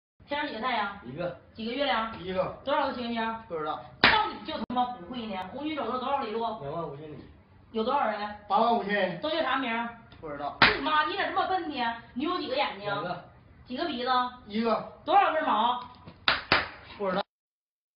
Le patron d'un restaurant corrige ses employés pour mauvaise conduite
S'il vous plaît cliquez sur Partager
Merci Bcp